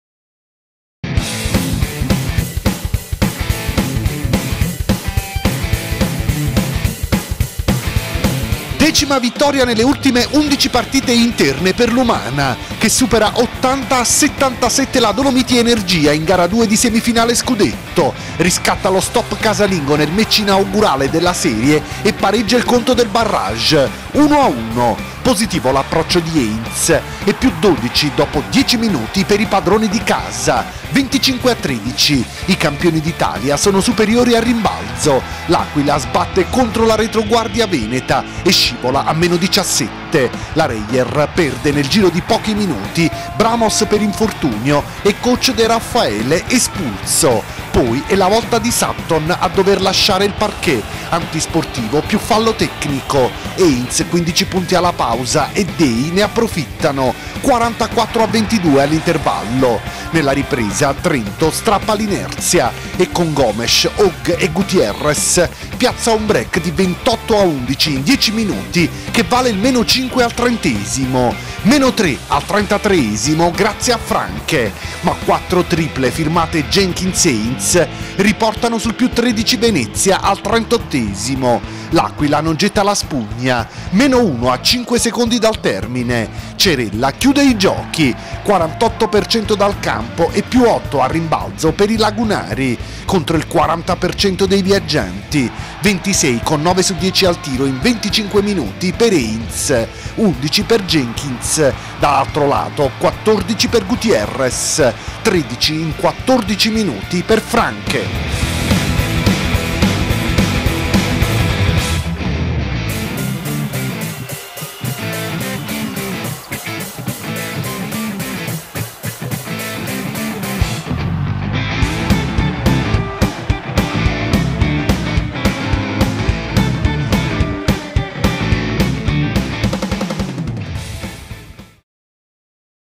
Highlights Gara2 Umana Reyer - Dolomiti Energia Trentino
Le azioni salienti del secondo atto della semifinale scudetto, serie ora in parità sull'1-1. Martedì 29 maggio alle 20.45 Gara3 alla BLM Group Arena di Trento.